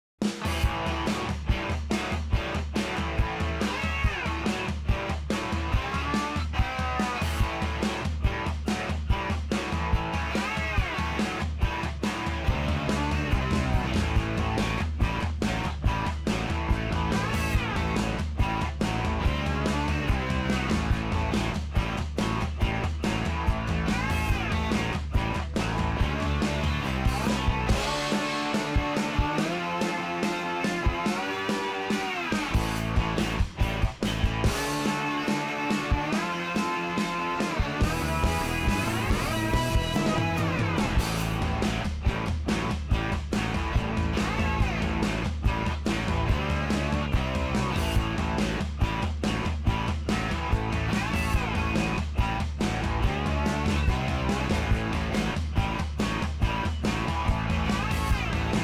Today is... Harald "Toni" Schumacher's birthday. 4#
Celebrate football legend Harald Schumacher's birthday with this thrilling compilation! Relive the magic of his best saves and unforgettable moments on the field. Happy Birthday, Harald!